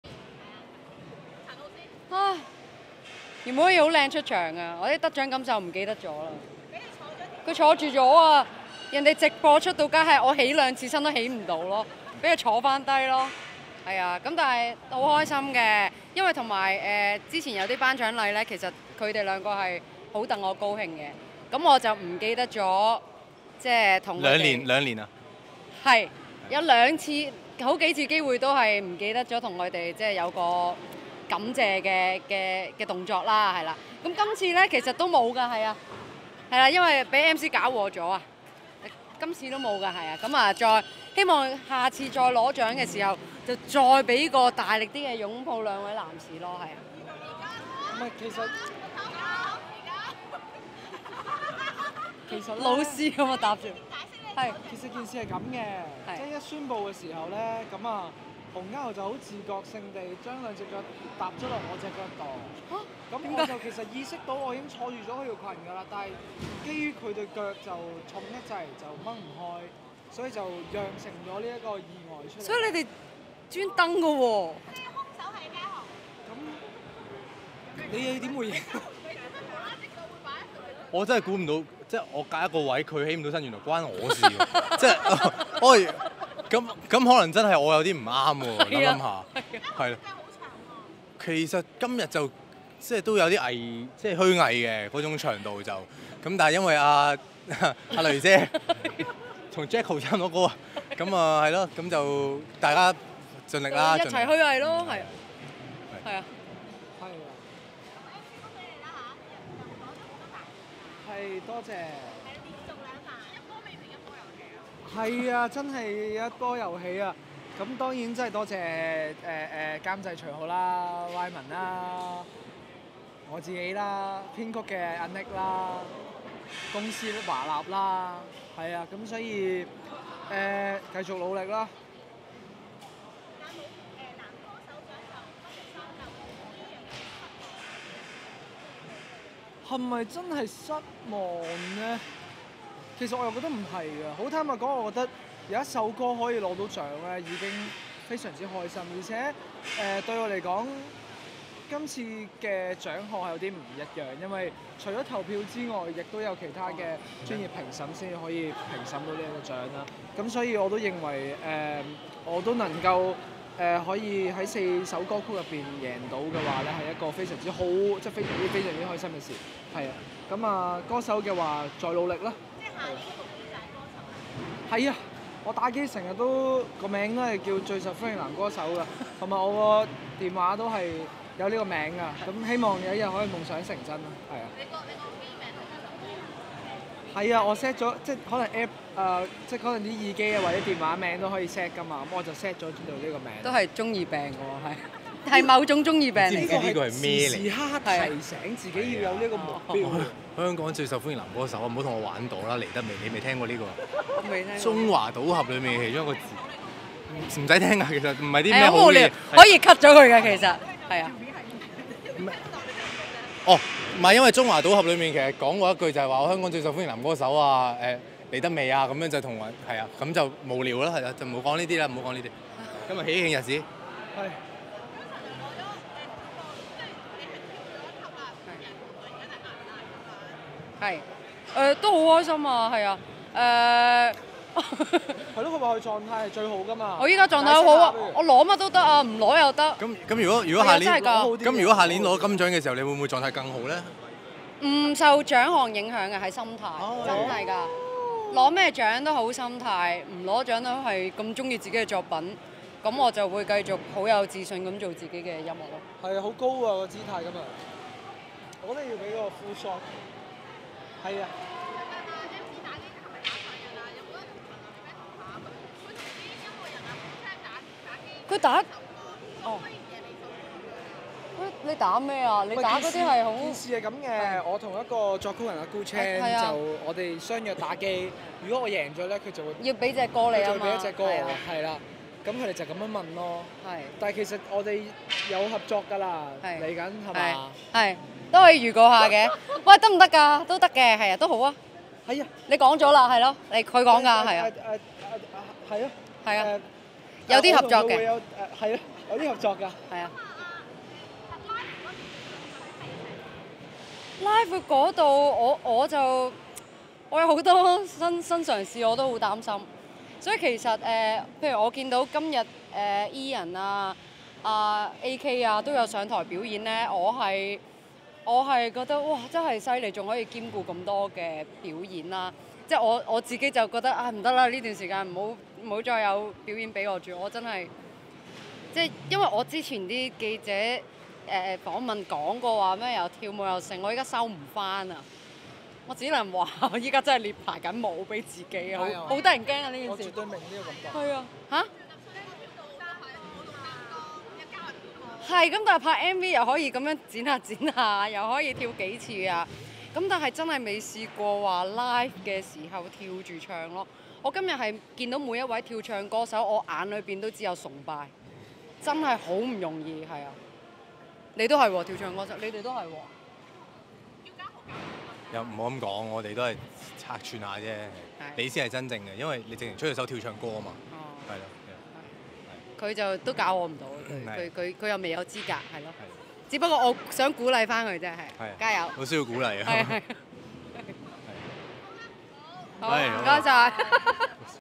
CHILL CLUB頒獎禮2023｜陳蕾嘆得獎無法靚爆出場 偕MC嘉豪受訪左擁右抱
ViuTV音樂頒獎禮《CHILL CLUB推介榜 年度推介22/23》假亞洲國際博覽館舉行，陳蕾近年成績有目共睹，今年奪得「年度女歌手」銀獎，事後她與同門的MC張天賦、洪嘉豪一同在後台受訪。MC與嘉豪傍住陳蕾受訪，「華納三寶」一聚埋自然笑料百出，陳蕾談起自己上台領獎一刻，長裙卻被旁邊的MC坐住，搞到她一度無法起身，場面搞笑。陳蕾訪問時先望MC然後再嘆一口氣，自言本身好靚站台，可是被MC坐住條裙，搞到她唔記得得獎感受，不過陳蕾就表示每次得獎，兩人都會戥她開心，表示下次若有機會得獎，會向兩位男士送上大大力的擁抱，現場記者隨即叫她現在立即送抱，陳蕾即時伸開雙手左擁右抱，之後MC再自爆跟陳蕾將有合作，可是現階段仍要賣關子。

bmgproductionmusic